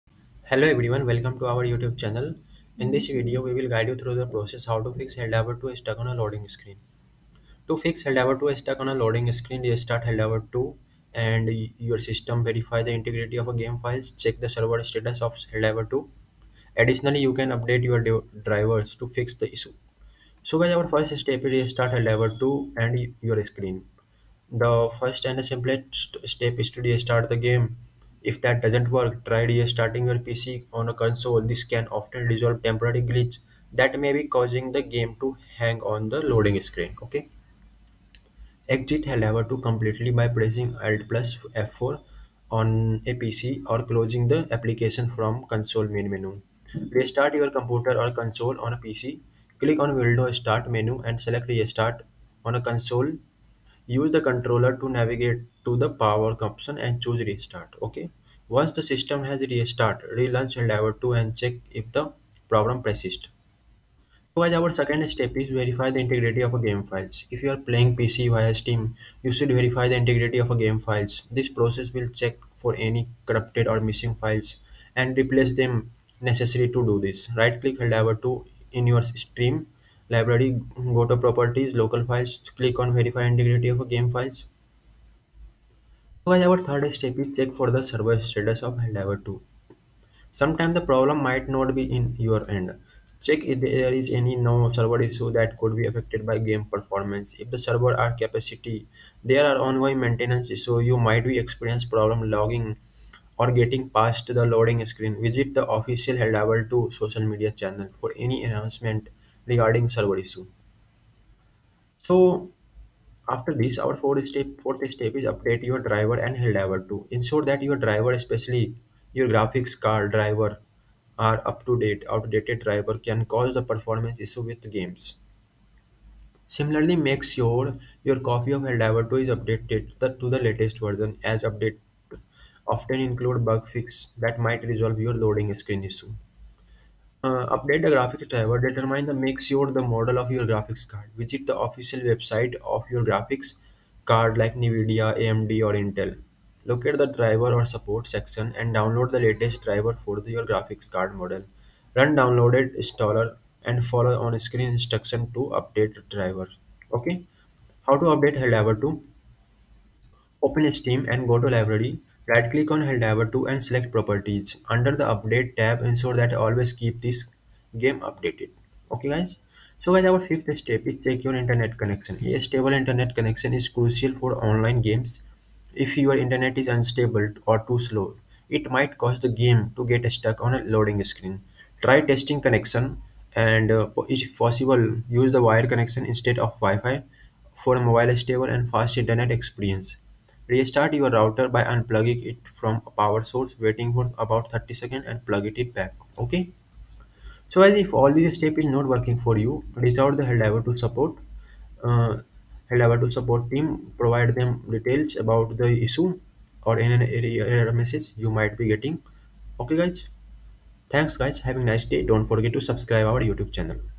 How To Fix Helldivers 2 Stuck On Loading Screen
in this video we will guide you through the process How To Fix Helldivers 2 Stuck On Loading Screen
To fix Helldivers 2 stuck on loading screen, restart Helldivers 2 and your system, verify the integrity of game files, and check the server status of Helldivers 2. Additionally, you can update your drivers to fix the issue

00:00  Introduction
00:21  Solution1:Restart Helldivers 2 And Your System
01:05  Solution2:Verify The Integrity Of Game Files (for PC)
01:55  Solution3:Check For The Server Status Of Helldivers 2
02:50  Solution4:Update Your Drivers And Helldivers 2
03:18  Solution5:Check Your Internet Connection
03:40  Solution:Reach Out To Helldivers 2 Support